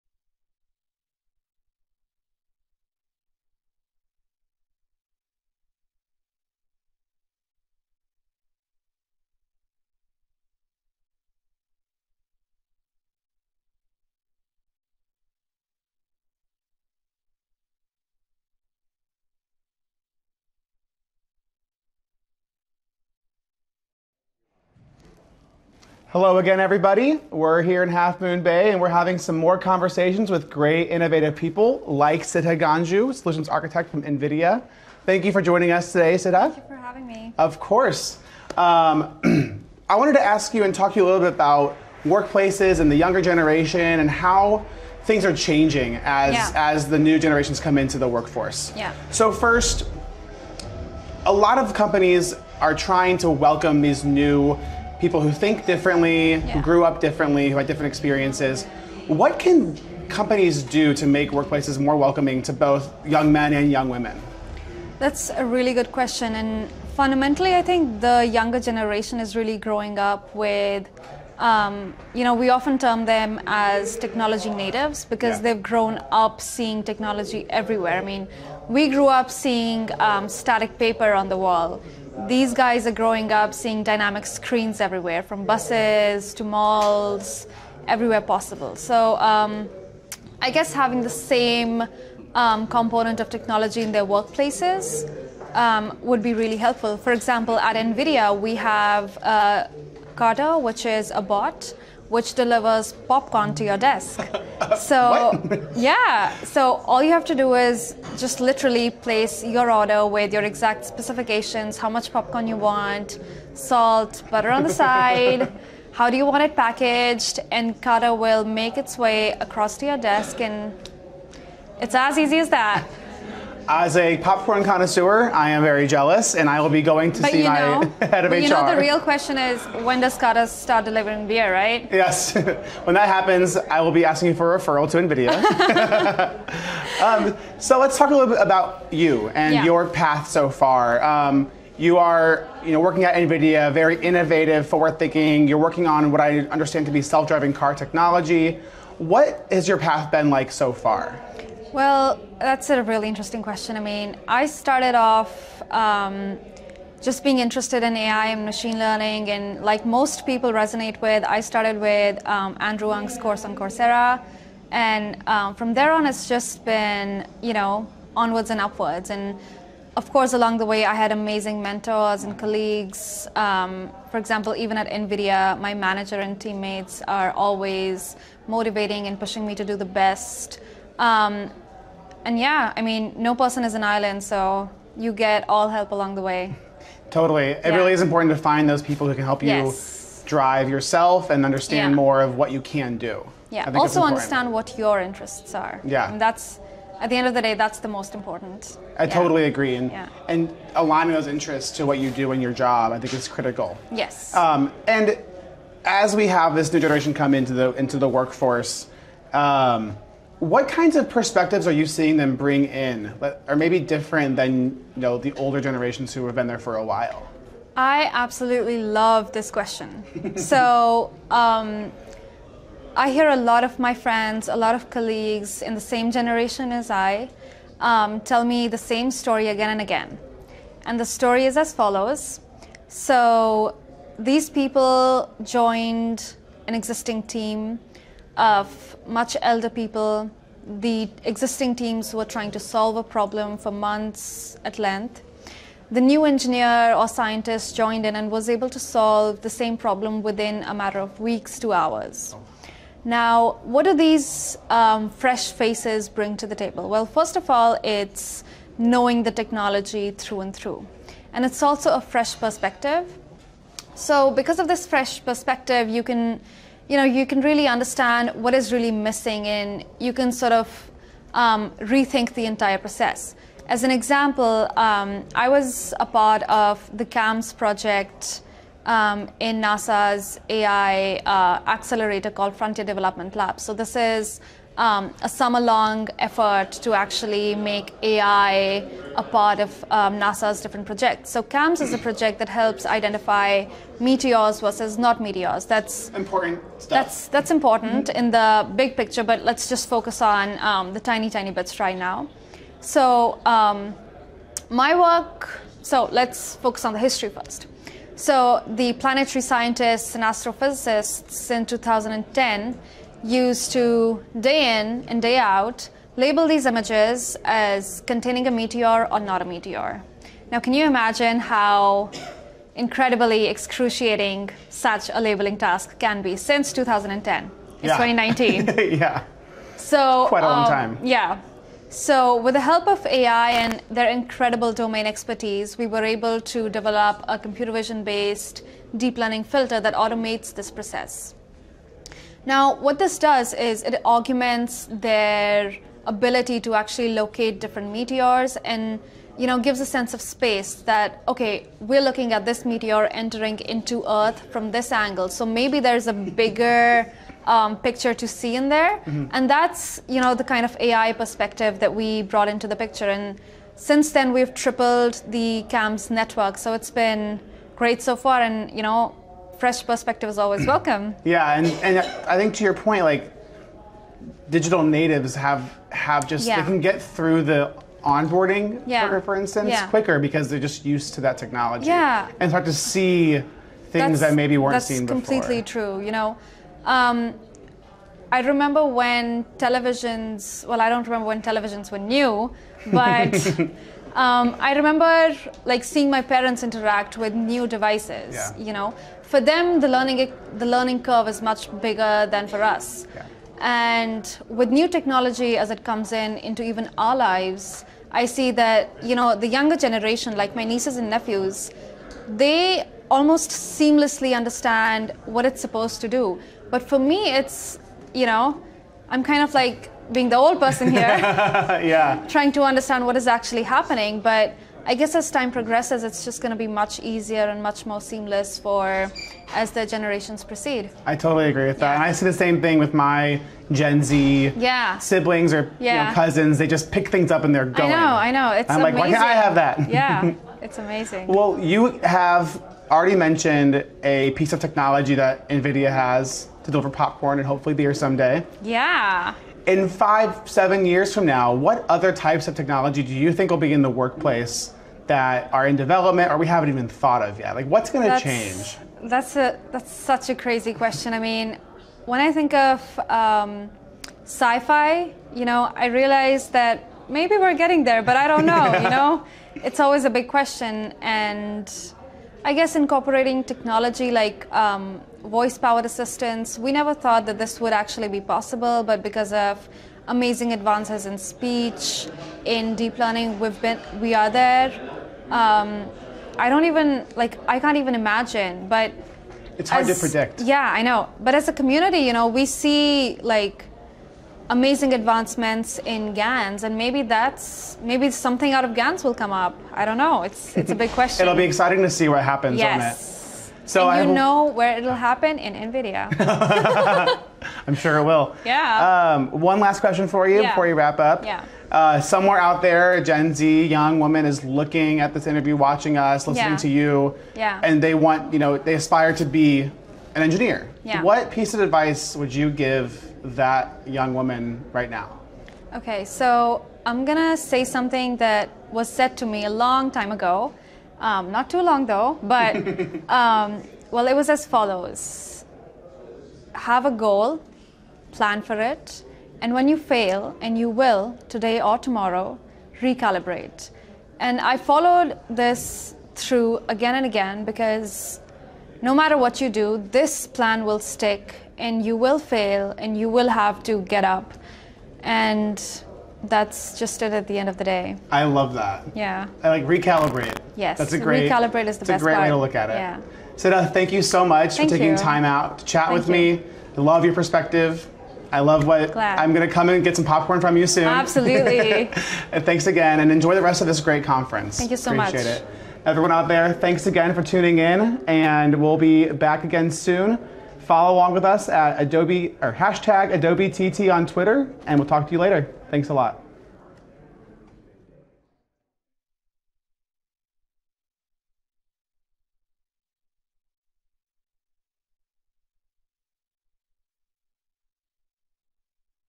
Adobe Think Tank: Creating Fulfilling Workplaces for Younger Generations | Adobe Acrobat
What new perspectives are millennials and Generation Z bringing to the world of work? Siddha Ganju shares her insights.